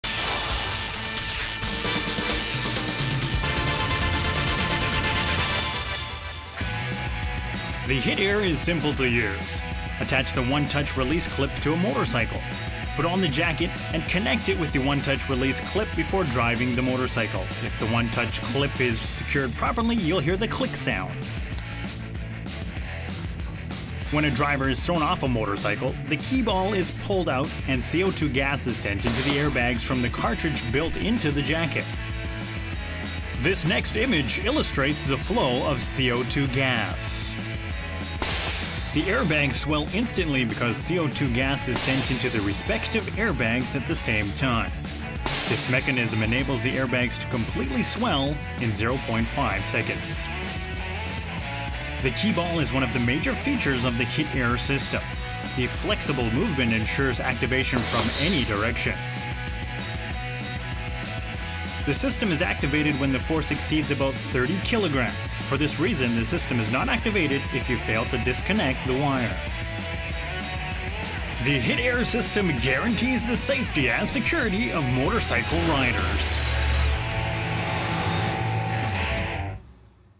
Hit Air Airbag jacket: How tos
Hot to wear a hit-air airbag jacket